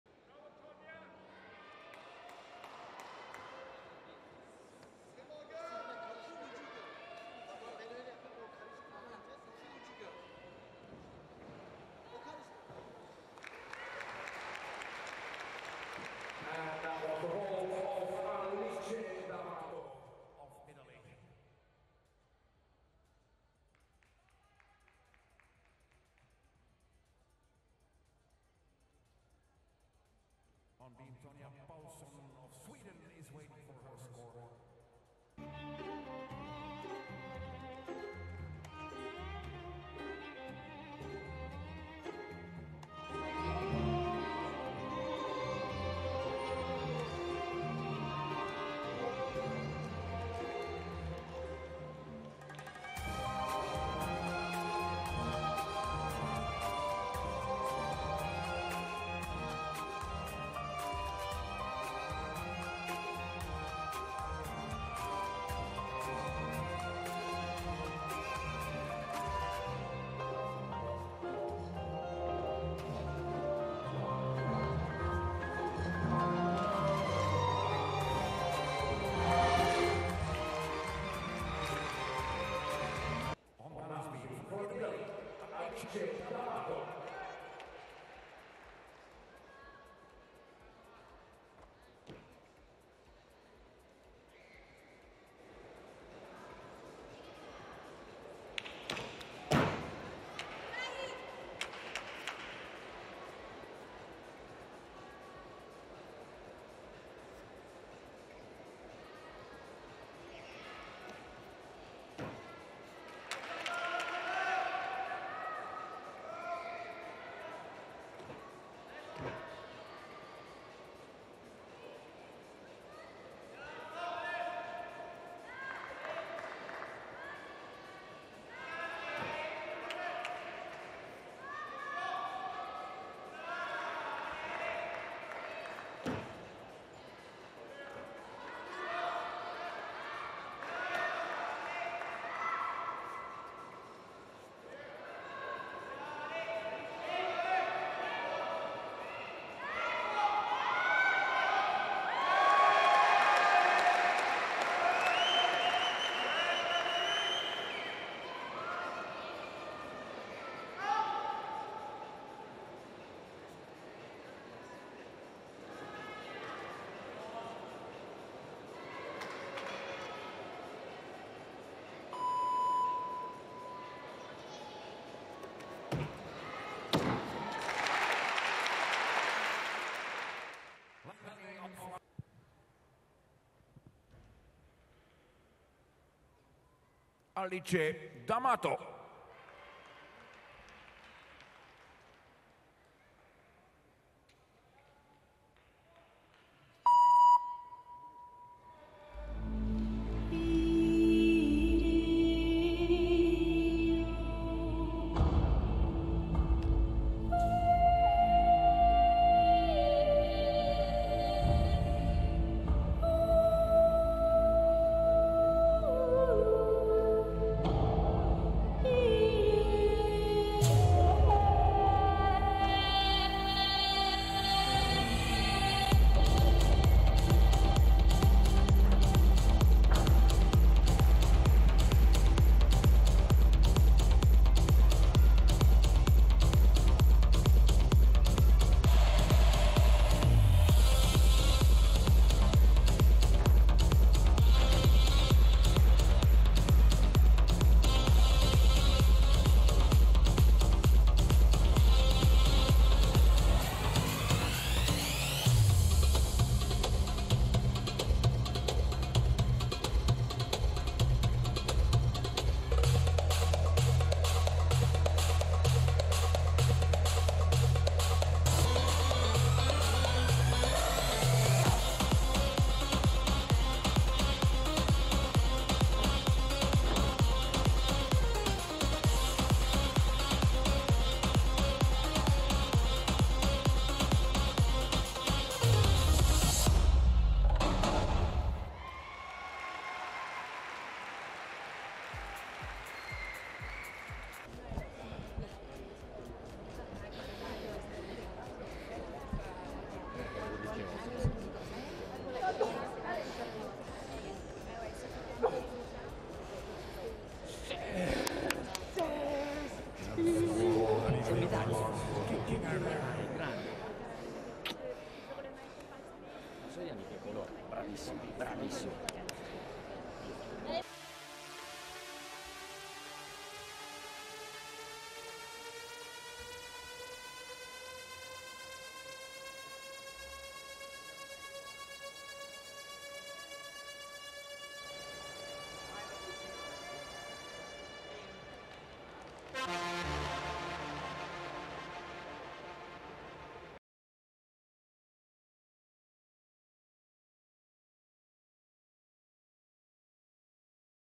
Alice D'AMATO (ITA) - 2023 Artistic European bronze medallist, all-around
10th European Men's and Women's Artistic Gymnastics Championships, Antalya (TUR), 11 to 16 April 2023

Alice D'AMATO (ITA) scored 54.500 in the all-around final... third place and bronze medal!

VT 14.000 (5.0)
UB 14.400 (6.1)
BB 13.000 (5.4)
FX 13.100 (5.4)